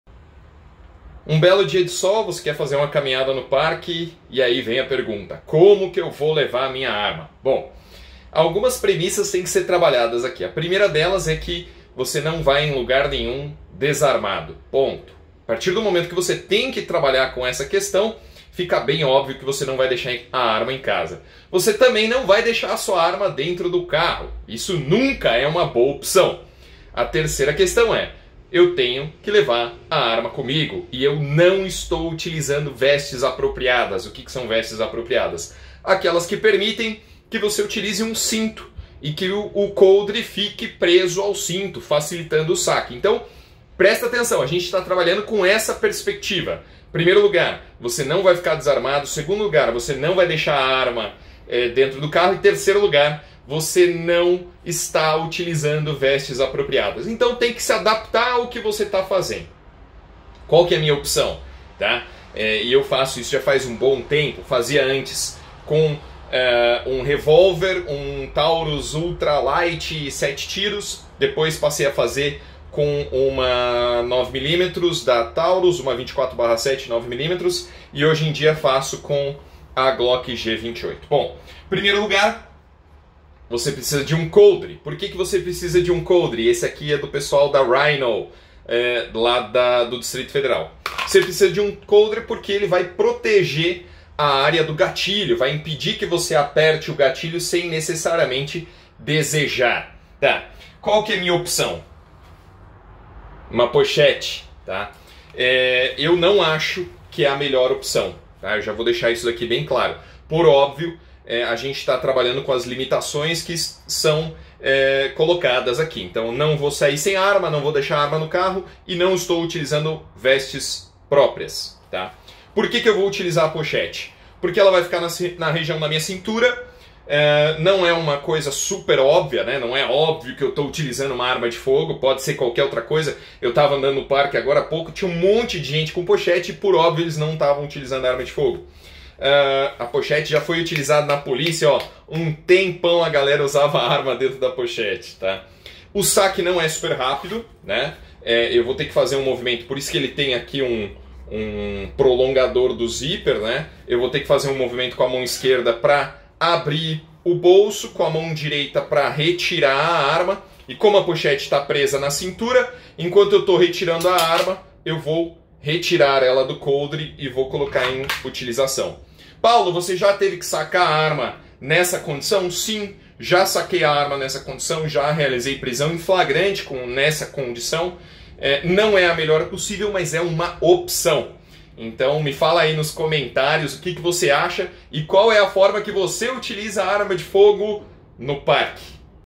Arma no parque?
Como portar uma arma se você não está usando roupas apropriadas?